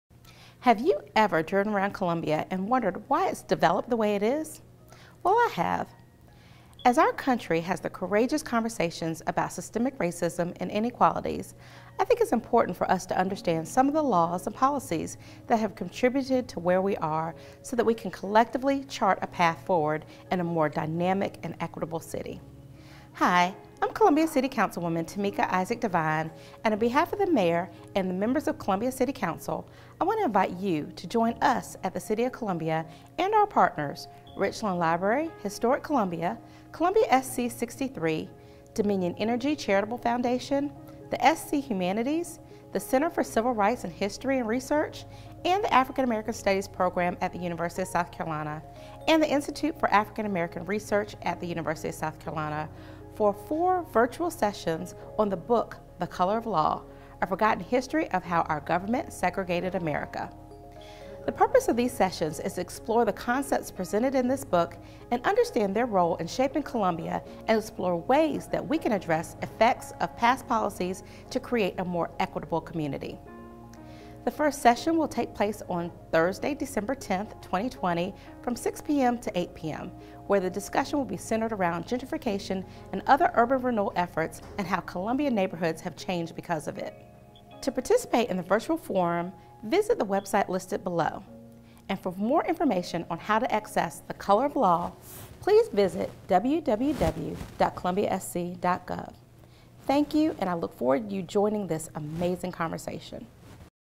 City of Columbia and Local Partners Present A Four Part Community Conversation on The Color of Law
The City of Columbia and local partners have worked together to host four virtual sessions on the book The Color of Law: A Forgotten History of How Our Government Segregated America. The purpose of these sessions is to explore the concepts presented in the book to understand their role in shaping Columbia and explore ways we can address the effects of past policies to create a more equitable community.
The first session will take place on December 10, 2020 from 6:00pm to 8:00pm, where the discussion will be centered around gentrification and other urban renewal efforts and how Columbia’s neighborhoods changed because of it.
Panelists for the first session, to be moderated by Warren Bolton, include:
Mattie Johnson Roberson, Former Ward One Resident, President of the Ward One Reunion Organization
Franchot Brown, Former Ward One Resident, Civil Rights Lawyer
Joe Darby, Former Wheeler Hill Resident, Civil Rights Activist
Rebecca Liebson, Reporter, The State
Stuart Andrews, Attorney, Civil Rights Activist
Ways to Access Copies for “The Color of Law"